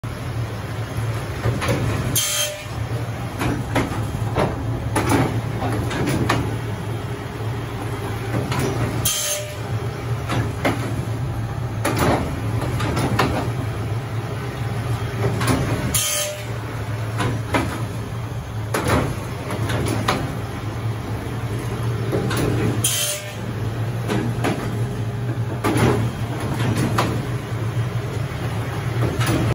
Welbond Perfect Binding Machine at Rajmudra Printers, Thane
This is a single clamp Perfect Binding Machine
You Have to print and gather pages as per sequence and Keep ready Book Cover with creasing asper the width of the book. At first stage operater inserts ready gathered pages in a clamp. At next stage Book cover is taken and at last stage ready book is delivered .